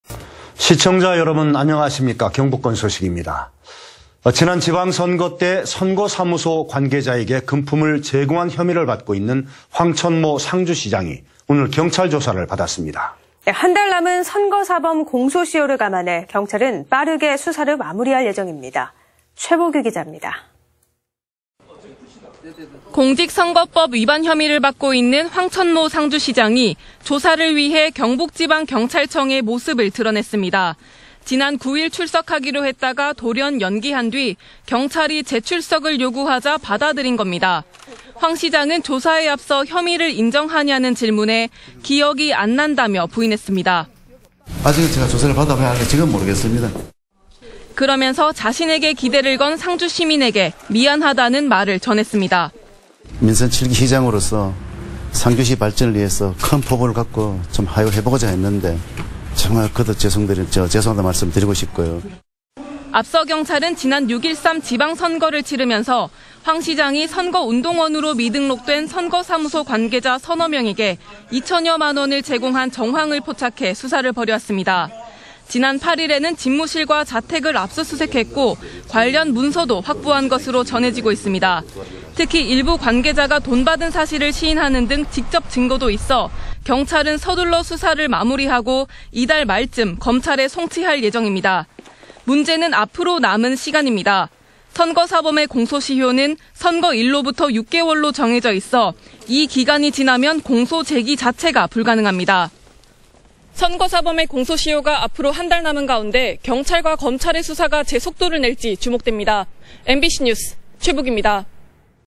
"금품 제공 기억 안 난다"..상주시장 경찰 소환조사 / 안동MBC
2018/11/12 15:26:24    작성자 : 최보규

◀ＡＮＣ▶
지난 지방선거때 선거사무소 관계자들에게
금품을 제공한 혐의를 받고 있는
황천모 상주시장이 경찰에 출석했습니다.

한 달 남은 선거사범 공소시효를 감안해
경찰은 빠르게 수사를 마무리할 예정입니다.

최보규 기자입니다.
◀ＥＮＤ▶

◀ＶＣＲ▶
공직선거법 위반 혐의를 받고 있는
황천모 상주시장이 조사를 위해
경북지방경찰청에 모습을 드러냈습니다.

지난 9일 출석하기로 했다가 돌연 연기한 뒤, 경찰이 재출석을 요구하자 받아들인 겁니다.

황 시장은 조사에 앞서 혐의를 인정하냐는
질문에 "기억이 안 난다"며 부인했습니다.

◀ＳＹＮ▶황천모 상주시장
"(금품 제공 전혀 기억이 안 나시는 거예요?) 조사를 받아봐야 알겠는데 지금은 모르겠습니다"

그러면서 자신에게 기대를 건 상주시민에게
미안하다는 말을 전했습니다.

◀ＩＮＴ▶황천모 상주시장
"민선 7기 시장으로서 상주시 발전을 위해서 큰 포부를 갖고 해보고자 했는데 정말 거듭 죄송하다는 말씀을 드리고 싶고요.."

앞서 경찰은
지난 6.13 지방선거를 치르면서 황 시장이
선거운동원으로 미등록된 선거사무소 관계자
서너 명에게 2천여만 원을 제공한 정황을
포착해 수사를 벌여왔습니다.

지난 8일에는 집무실과 자택을 압수수색했고
관련 문서도 확보한 것으로 전해지고 있습니다.

특히 일부 관계자가 돈 받은 사실을
시인하는 등 직접 증거도 있어
경찰은 서둘러 수사를 마무리하고
이달 말쯤 검찰에 송치할 예정입니다.

문제는 앞으로 남은 시간입니다.

선거사범의 공소시효는 선거일로부터
6개월로 정해져있어, 이 기간이 지나면
공소 제기 자체가 불가능합니다.

(s/u) 선거사범의 공소시효가 앞으로 한 달 남은 가운데 검찰과 경찰의 수사가 제 속도를
낼지 주목됩니다.

MBC 뉴스 최보규입니다. (영상취재 최재훈)